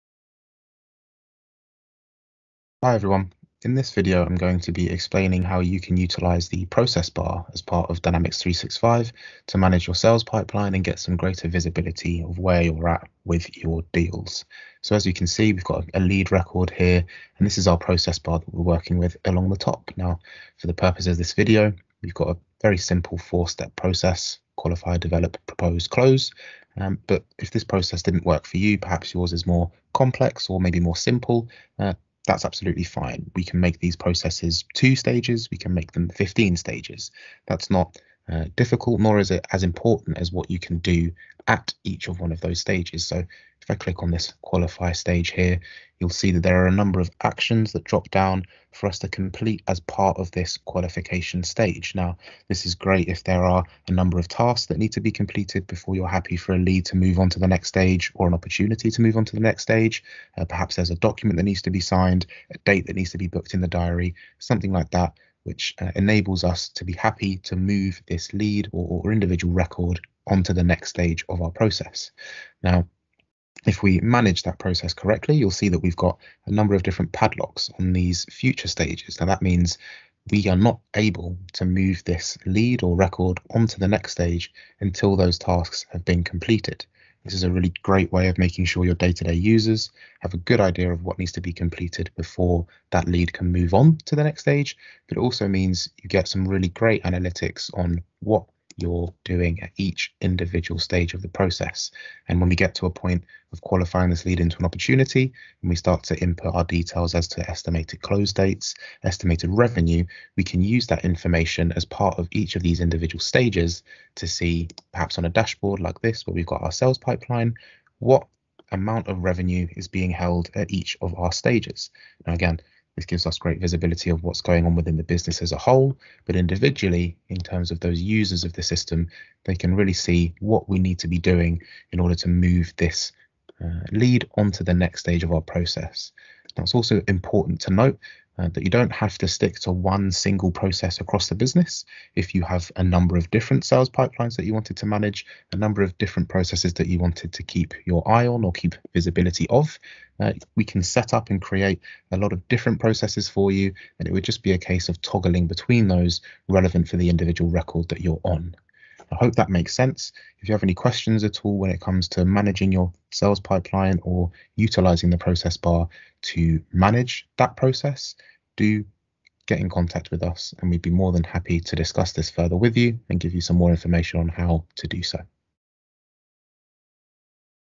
Microsoft Dynamics 365 - Managing your Sales Pipeline using the Business Process Bar -Cloud9 Insight
See into conversations and spot where a deal is at risk. Then use Dynamics 365’s coaching tools to help your colleagues take action to address any issues.